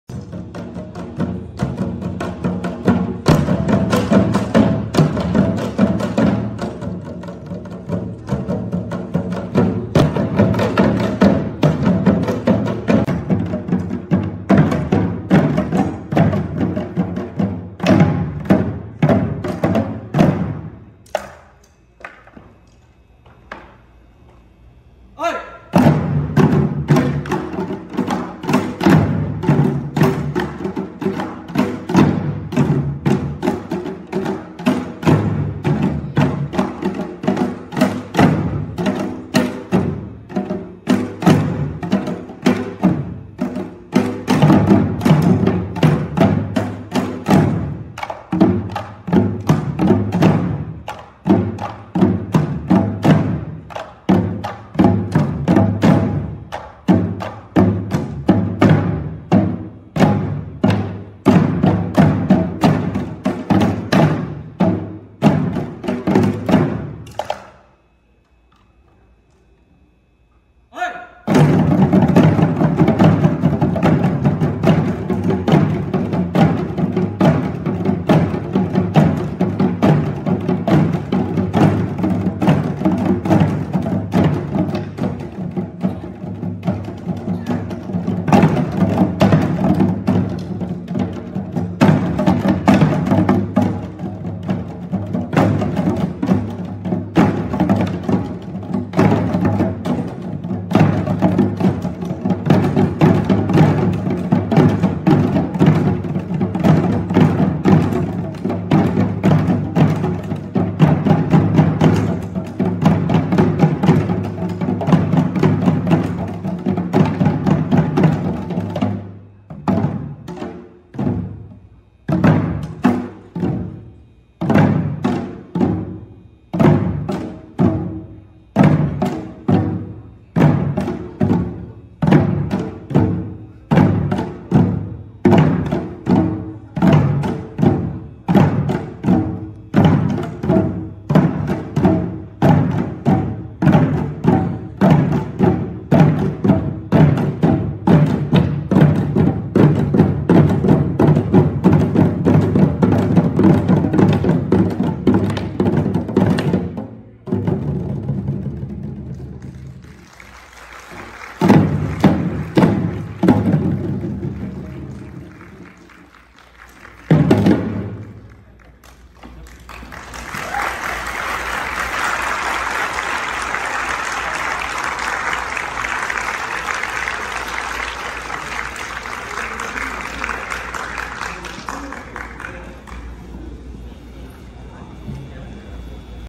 [Selahart] Janggu 장구북 | 2022 Central PA Korean Association Chuseok Event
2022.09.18 Selahart Group performed Janggu 장구북 (Korean traditional drum)  at Penn Harris Hotel in Camp Hill, PA to celebrate the Korean Chuseok (Harvest) Festival.

Performers:
Jungi, Rachel, Yongmin, Yongchan, Grace, Gyeongjin, Sophia